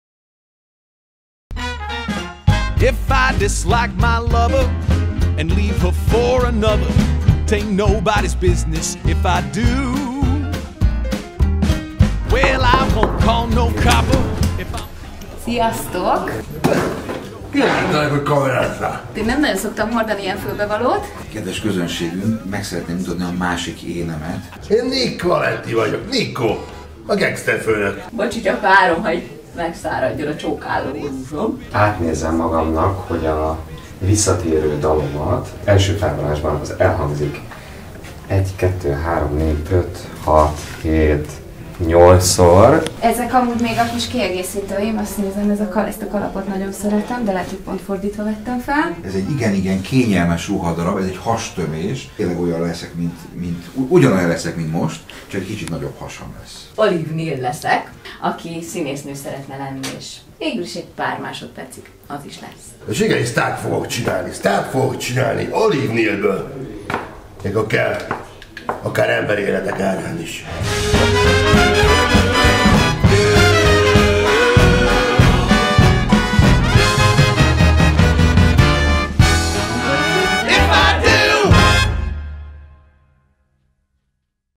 Lövések a Broadwayn - Backstage videó
Találkozz kedvenceiddel! Ha például kíváncsi vagy rá, hogy Ember Márk hogyan tanult meg szteppelni, Szinetár Dóra milyen „mintákat” használt egy idősebb, kiégett díva megformálásához, Szabó Győzőhöz miért áll közel a gengszter stílus, gyere! Október 13-án, 14-én és november 10-én, 11-én – a Lövések a Broadwayn című előadásunk után – egy jó hangulatú, kötetlen beszélgetés keretében, színészeink közreműködésével adunk bepillantást a kulisszák mögé!